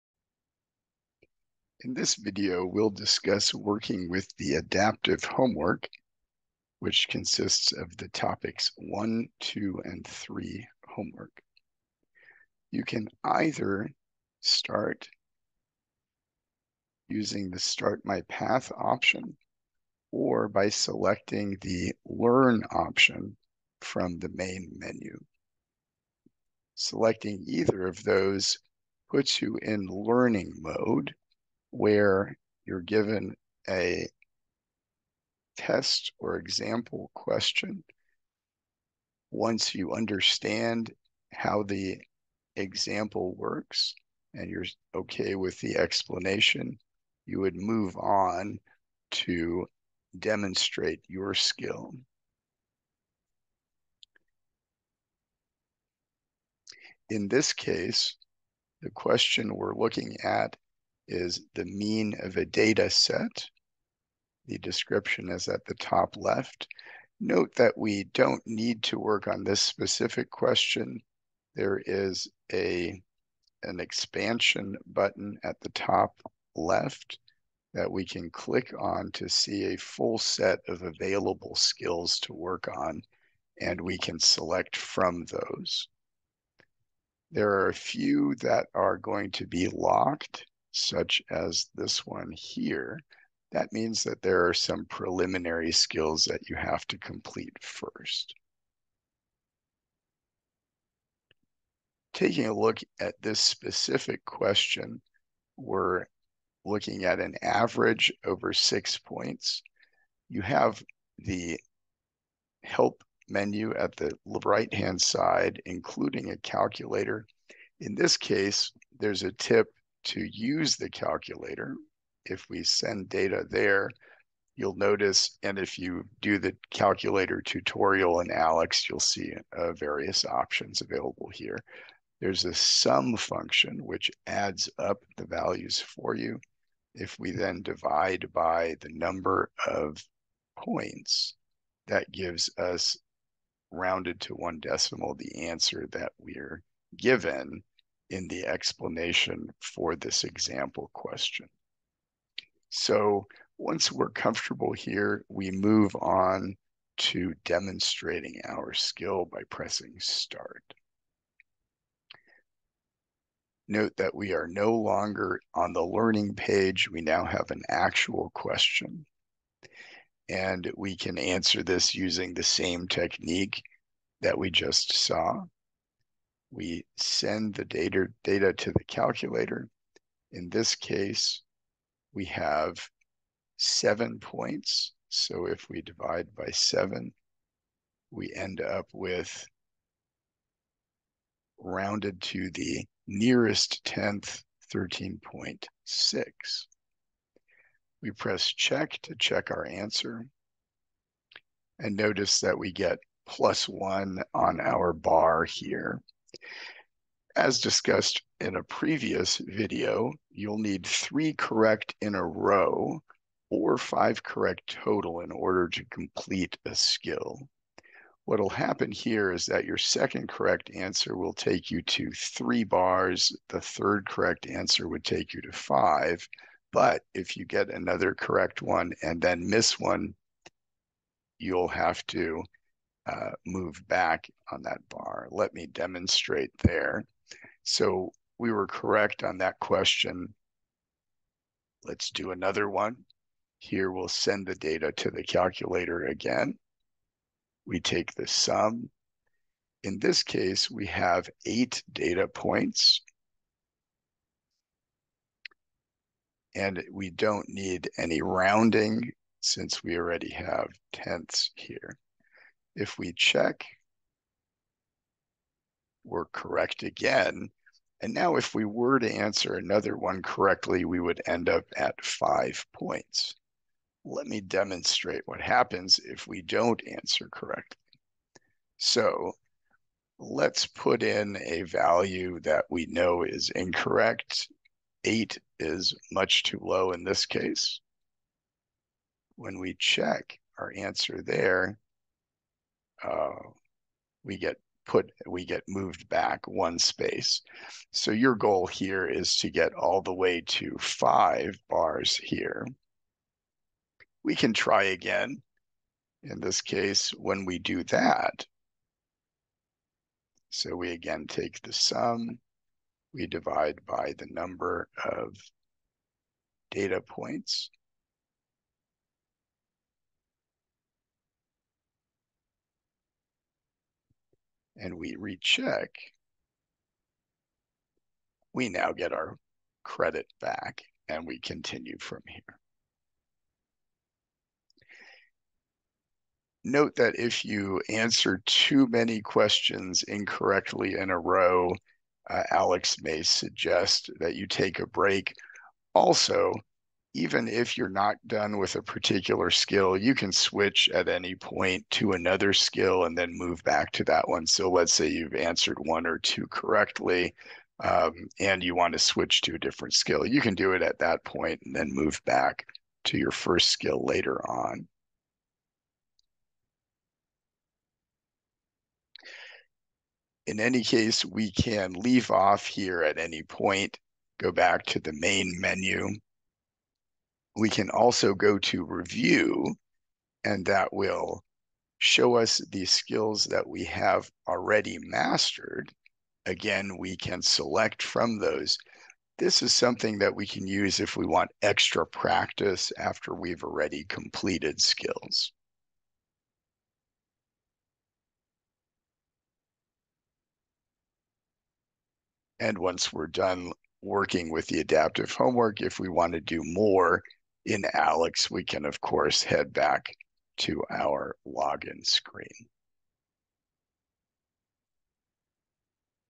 5 Working With Adaptive Homework
Discusses how to access and work on adaptive homework assignments 1-3